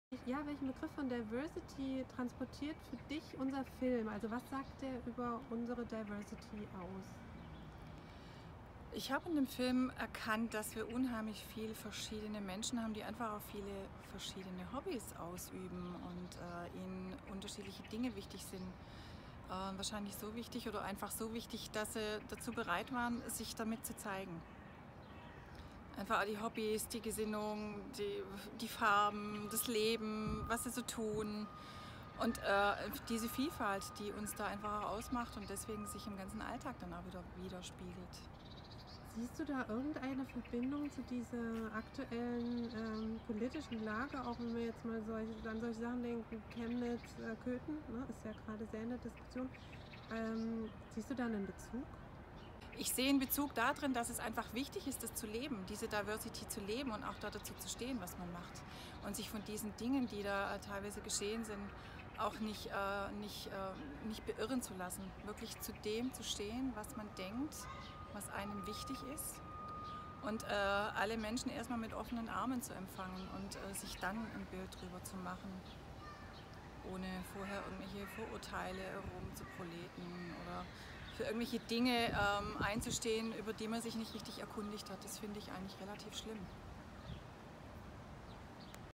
Wir sind divers und wirsindmehr - Daniela
Wir haben unsere Mitarbeiter gefragt, welche Aussage unser Clip "pme Bunte Vögel" für sie trifft und ob sie einen Zusammenhang zur aktuellen, innenpolitischen Lage sehen (Stichwort Chemnitz). Und hier seht ihr unseren Clip "Bunte Vögel" nochmal der von der @Charta der Vielfalt zur "Schönsten Diversity Aktion 2018" gekürt wurde.